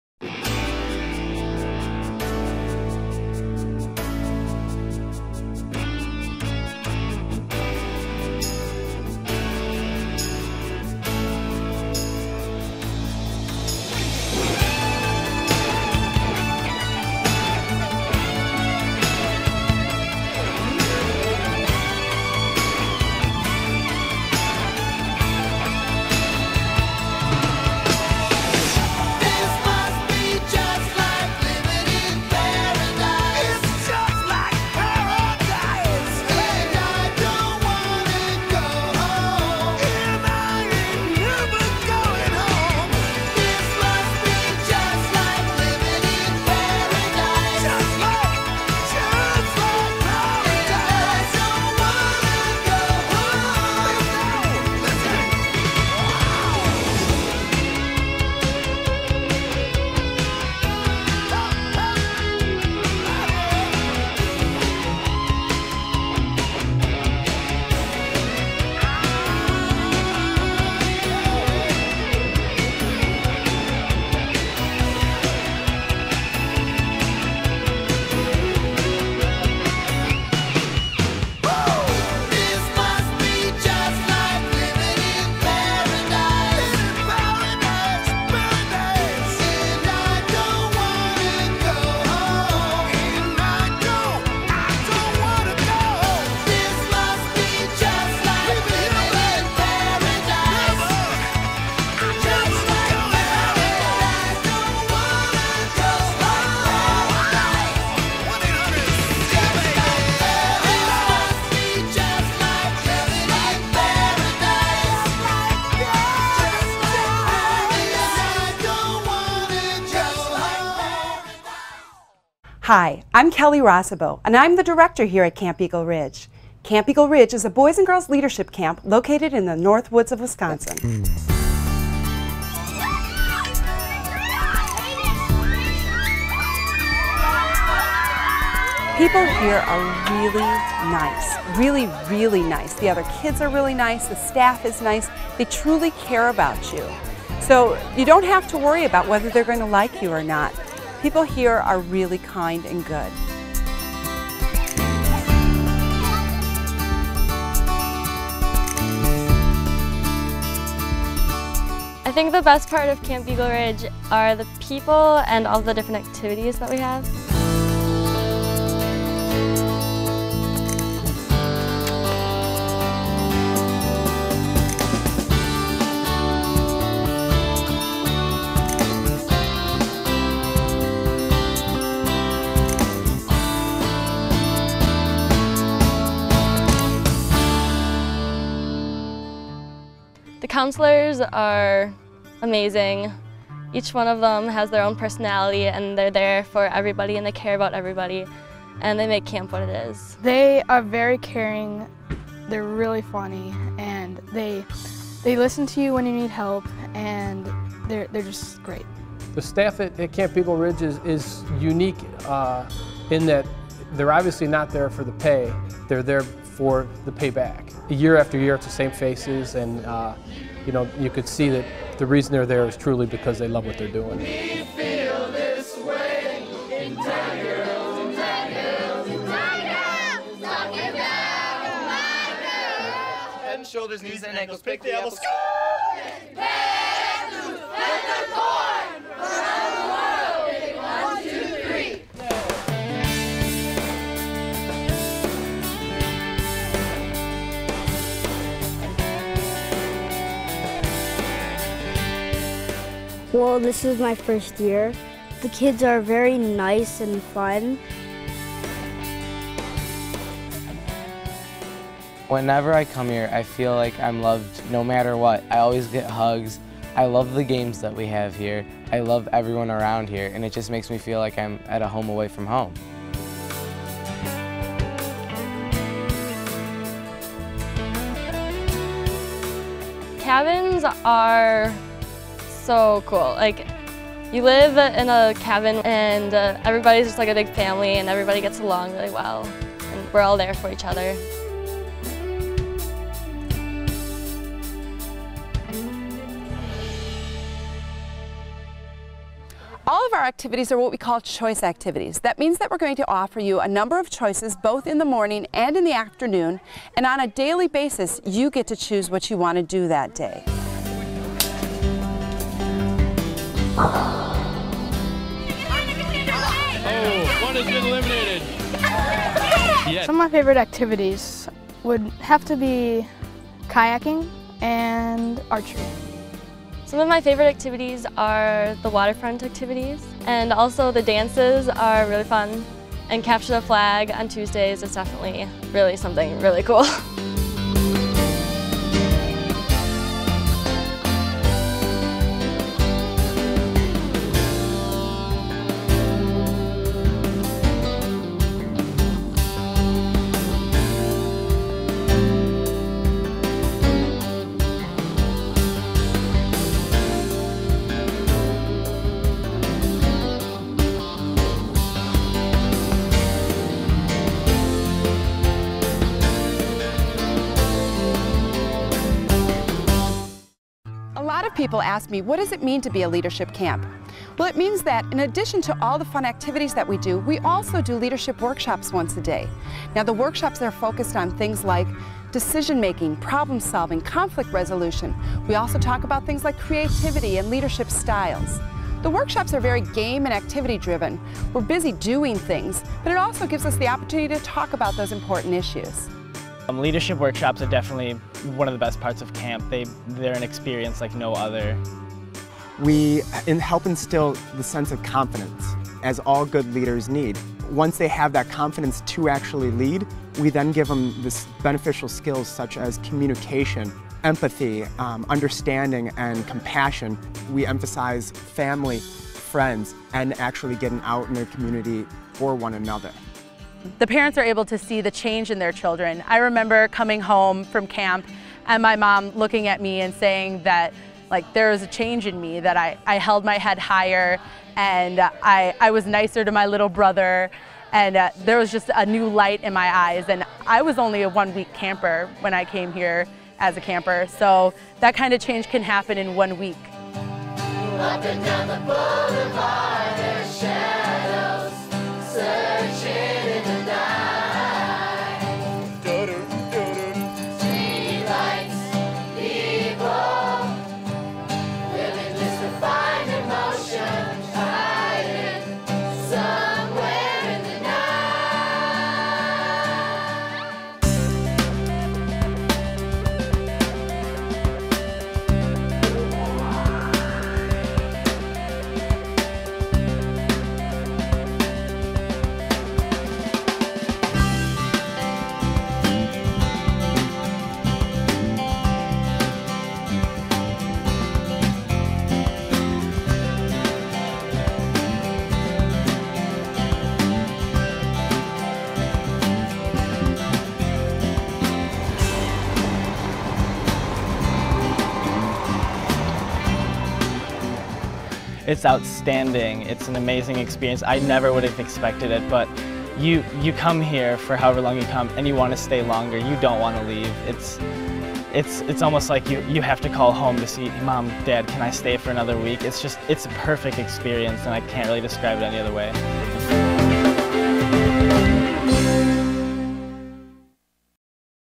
Camp Eagle Ridge - About Camp
Learn all about Camp Eagle Ridge, a boys and girls leadership camp located in the North woods of Wisconsin.